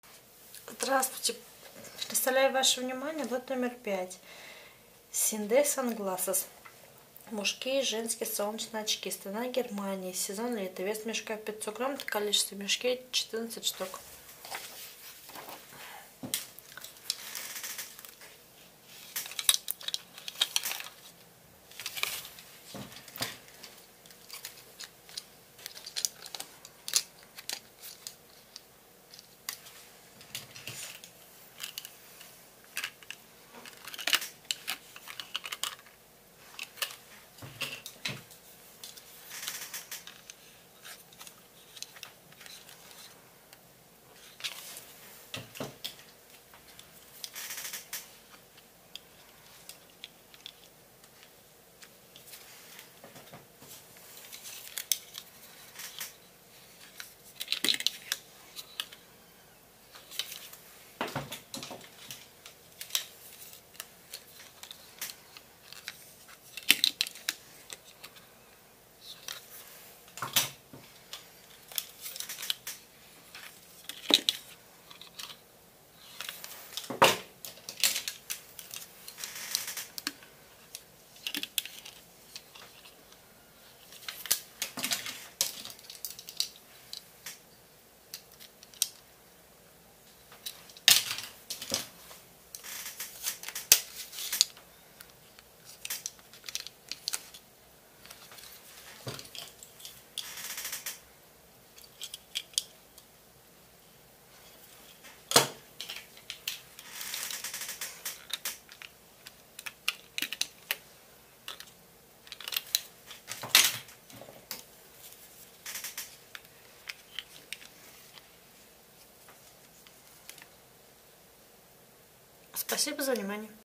C&A SUNGLASSES 5,сток одежда оптом
C&A SUNGLASSES (Лот№5)
Мужские и женские солнечные очки(сток)
Страна: Германия
Сезон: Лето
Цена за кг.: 14.51 евро
Цена за мешок: 7.25 евро
Вес мешка, кг.: 0.5